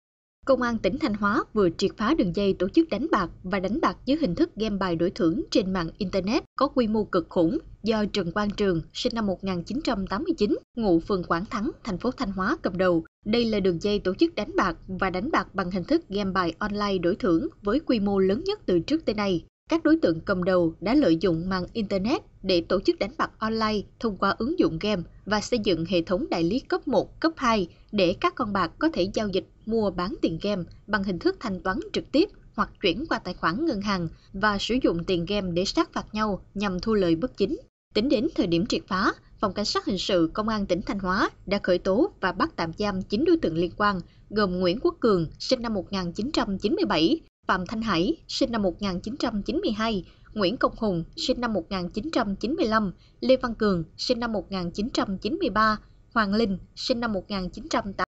Thanh Hóa: Phá đường dây đánh bạc 4.000 tỉ đồng qua mạng Internet | NLĐTV
Công an tỉnh Thanh Hóa vừa triệt phá đường dây tổ chức đánh bạc và đánh bạc dưới hình thức game bài đổi thưởng trên mạng Inetrnet có quy mô cực "khủng" do Trần Quang Trường (SN 1989, ngụ phường Quảng Thắng, TP Thanh Hóa) cầm đầu. Đây là đường dây tổ chức đánh bạc và đánh bạc bằng hình thức game bài online đổi thưởng với quy mô lớn nhất từ trước tới nay.